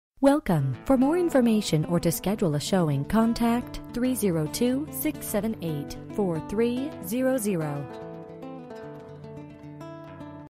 Homes for sale - 4581 DUPONT HWY, Townsend, DE 19734
L-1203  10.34 acres for sale, no builder tie in located in appoquinimink School District

Bedrooms: 0
Bathrooms: 0.00 Bathroom
Square Feet: 0
Price: $139,900

03/19/2017 07:17:01 am

Last modified: